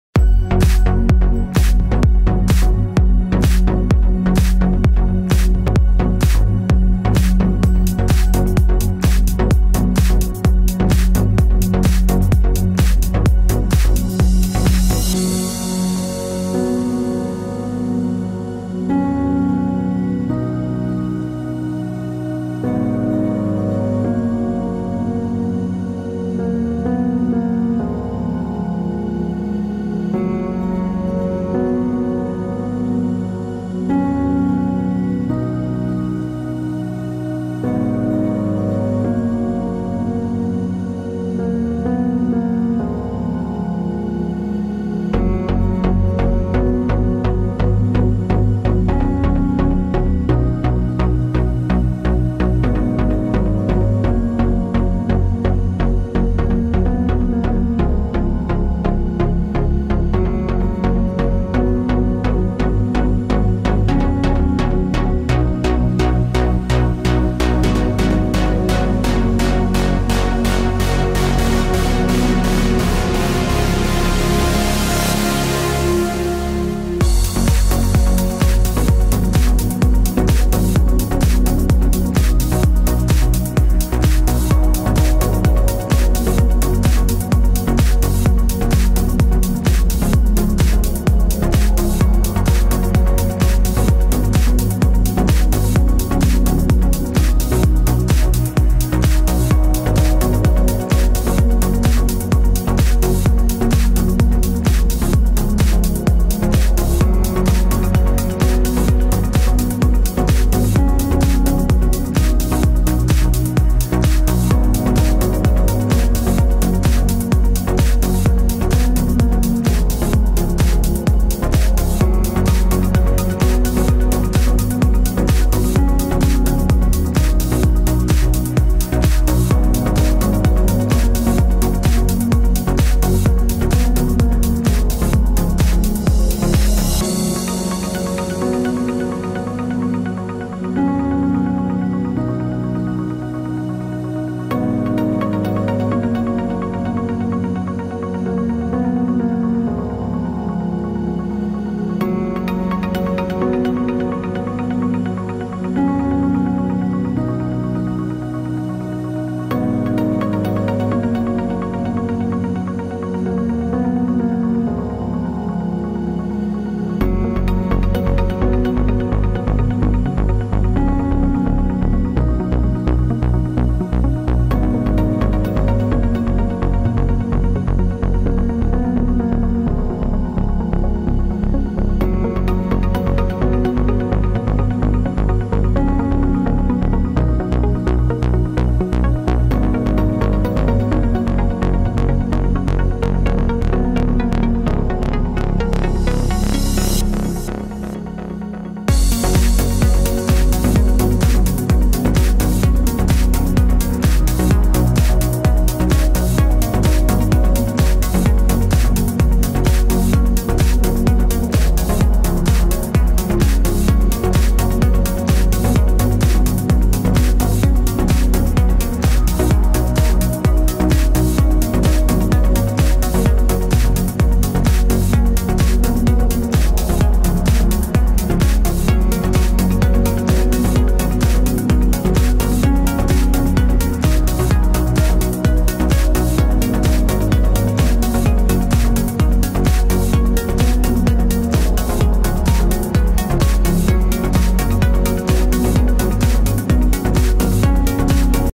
Como llegar a Pitayass Xela desde Calle Rodolfo Robles
Sigue el recorrido para llegar al mejor club de Quetzaltenango desde la calle Rodolfo Robles. ¡Te esperamos en tu segunda casa Pitayass Hotel, Club & Spa Xela!